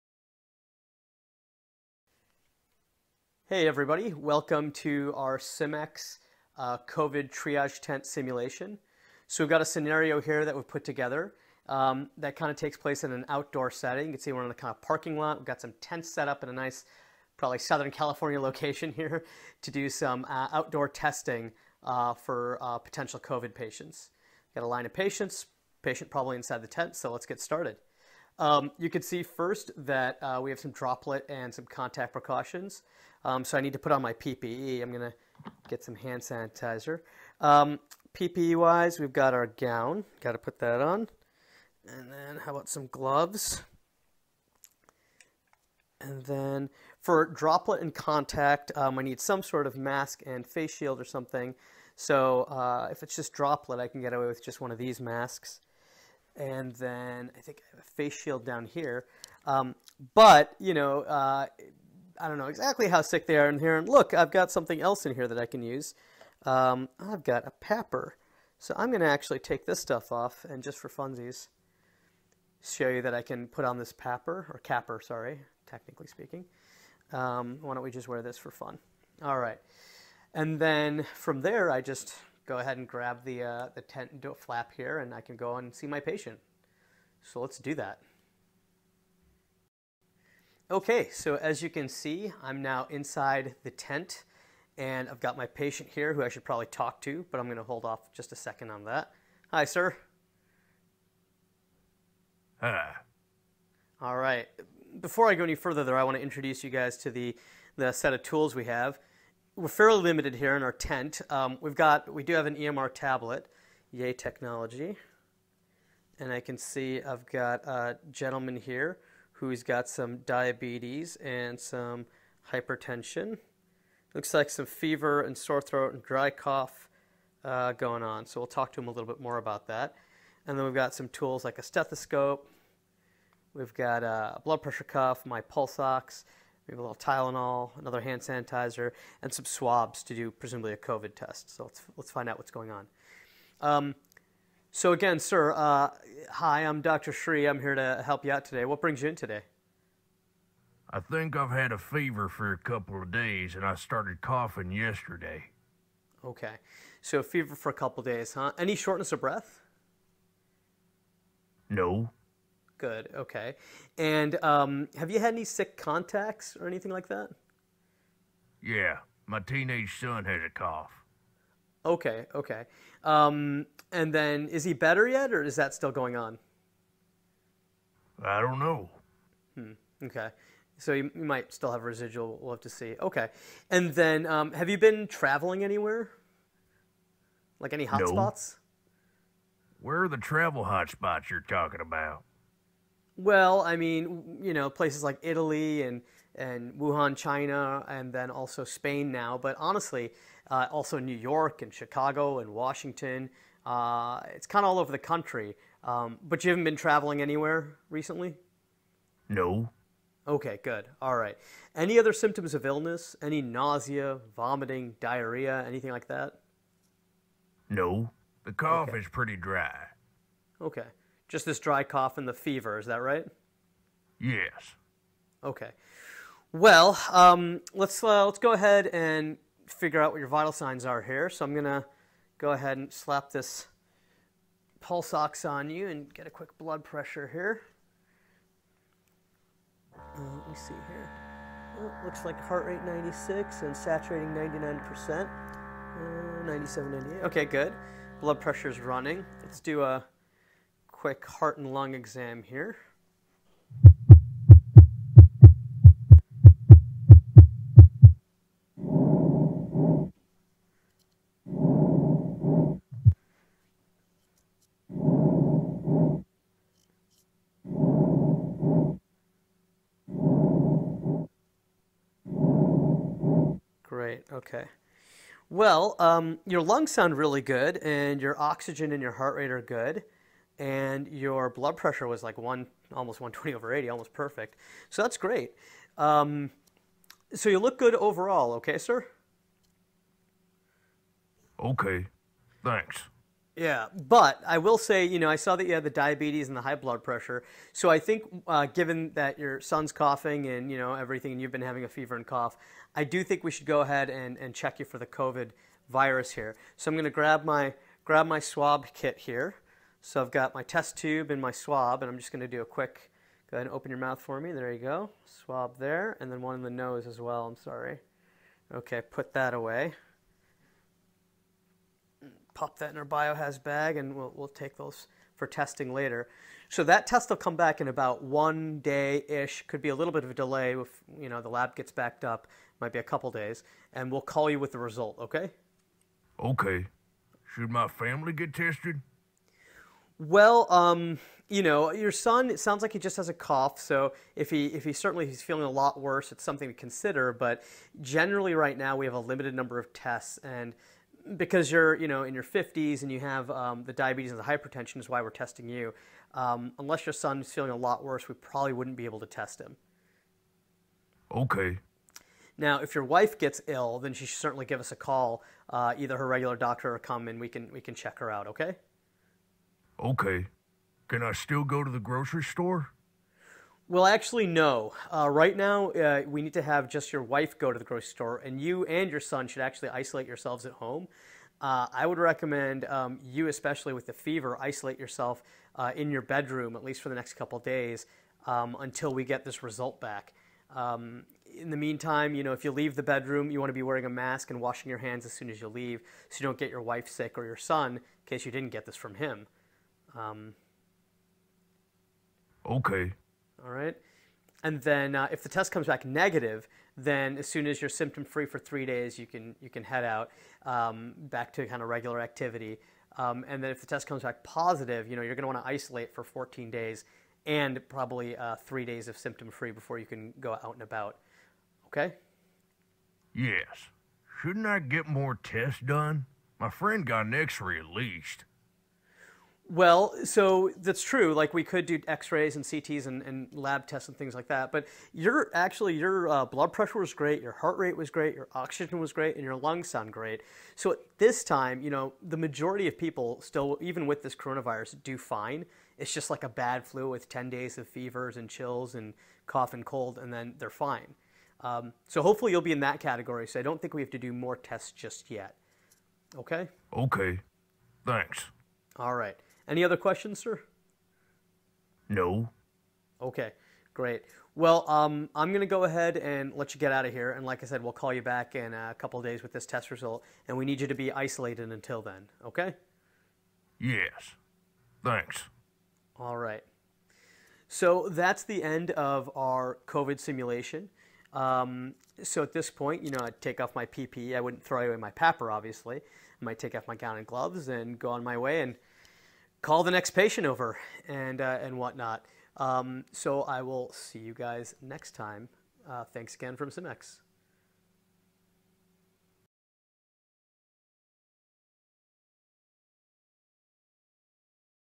COVID-19 Triage
A full walkthrough of SimX's VR COVID-19 evaluation scenario, for VR medical simulation training of healthcare providers.  This scenario is available for use free of charge to any healthcare organization; more information is available